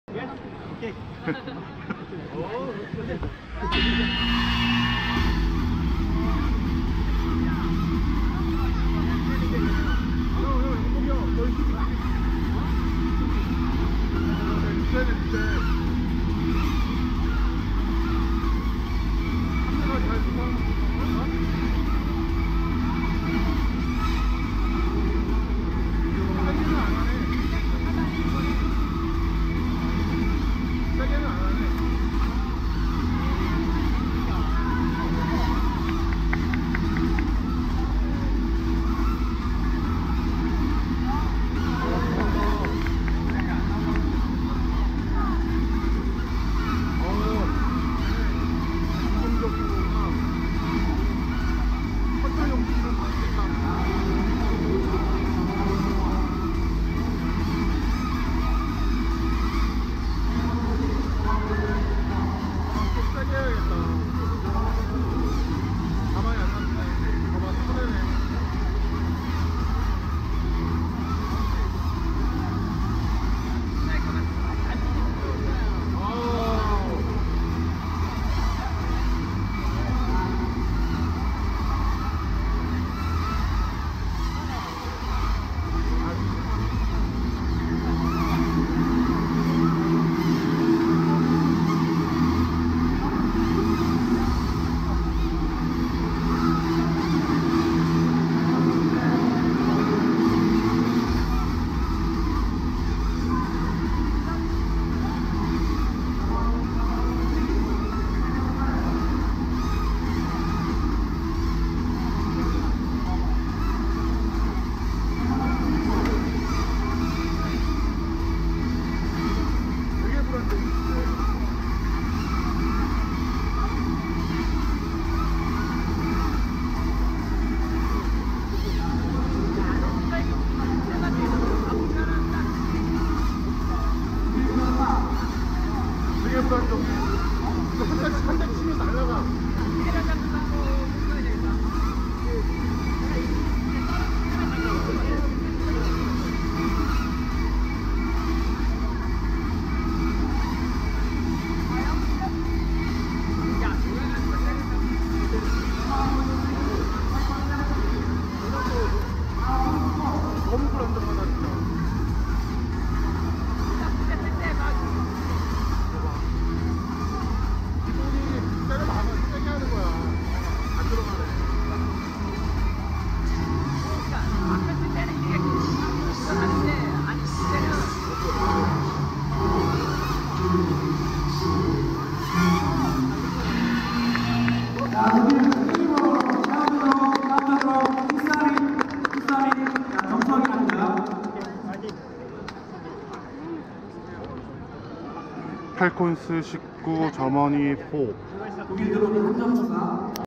독일(Germany) VS 팰콘스(예선) | 제1회 경북도지사배 국제드론축구대회
2019년 9월 29일 토요일 김천실내체육관에서 있었던 제1회 경북도지사배 국제드론축구대회 예선경기 영상입니다.
촬영,편집: 필름빅터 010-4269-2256